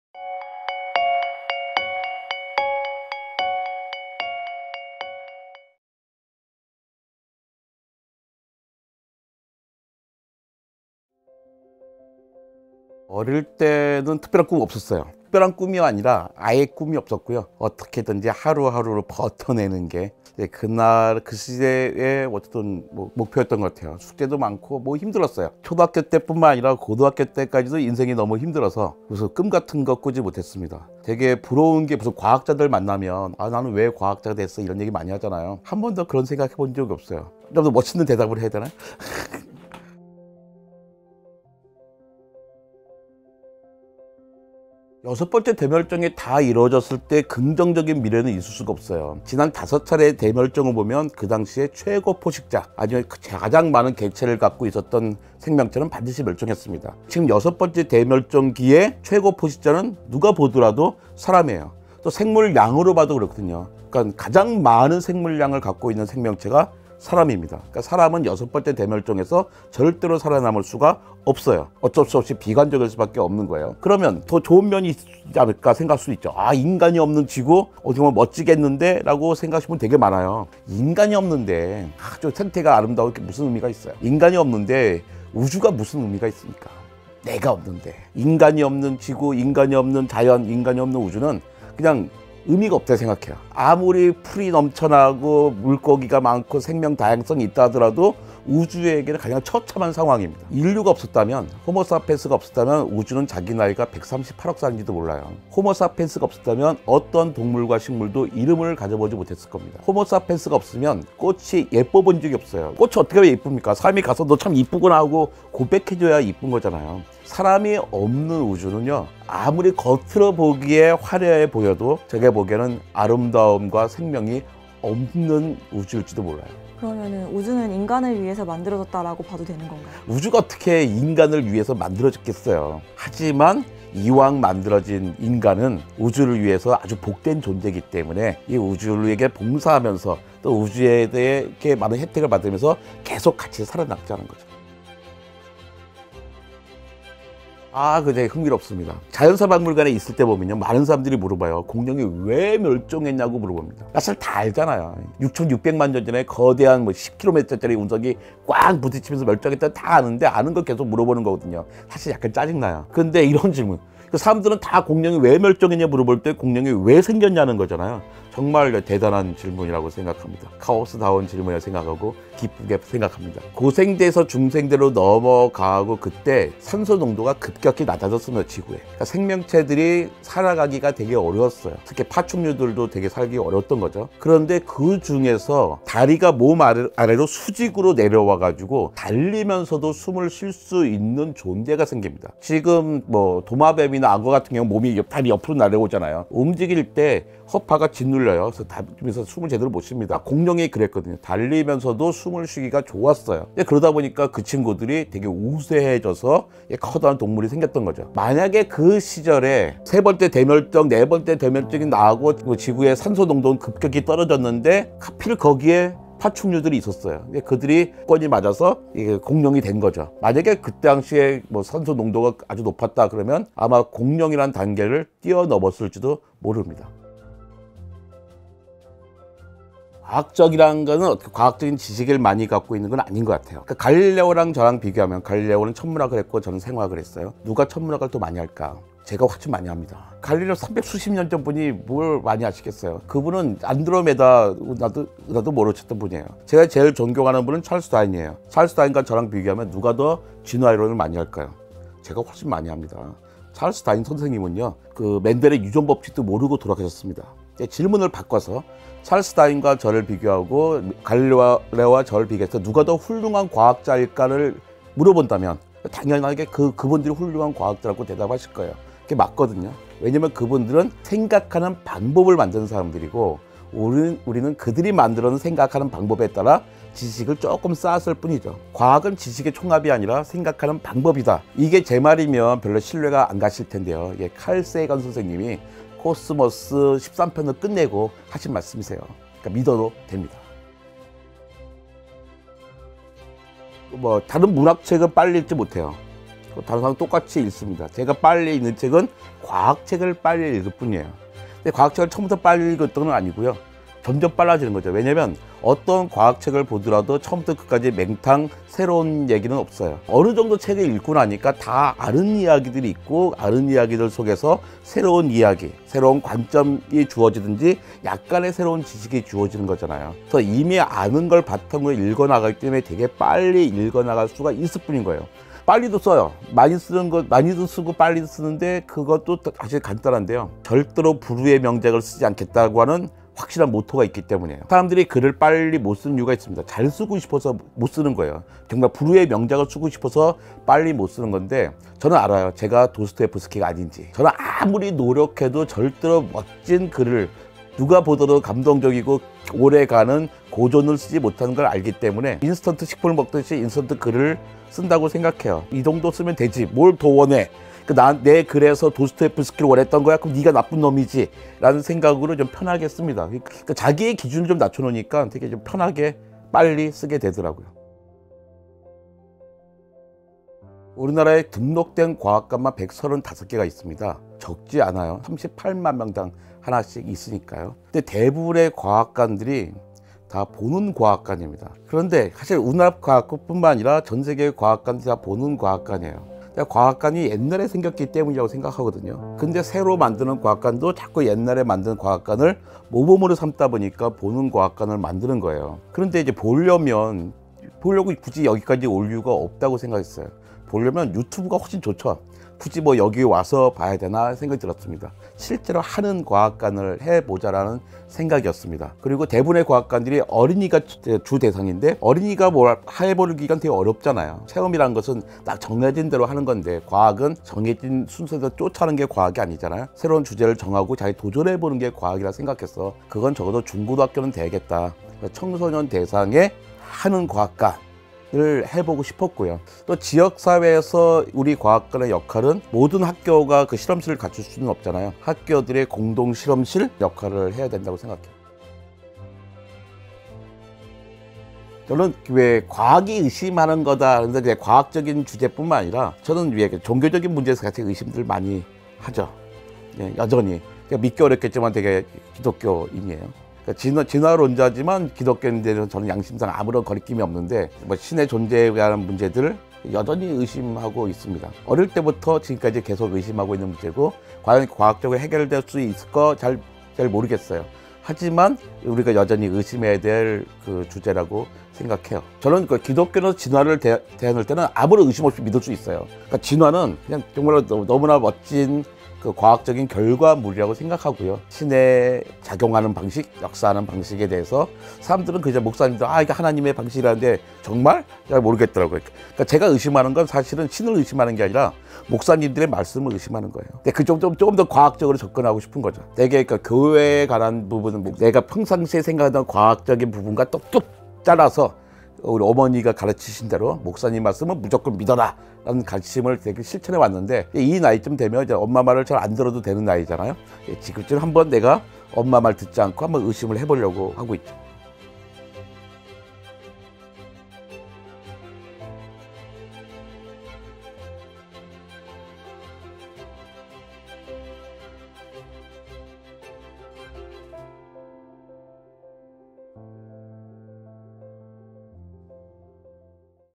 [석학인터뷰] 이정모_ 인류가 없는 우주? 우주에게는 가장 처참한 상황(수정)
[석학인터뷰] 인류가 없는 우주? 우주에게는 가장 처참한 상황 |  이정모 서울시립과학관장

자막의 오타를 수정하여 재업로드합니다.
제보해주신 탁건이님 감사합니다:)!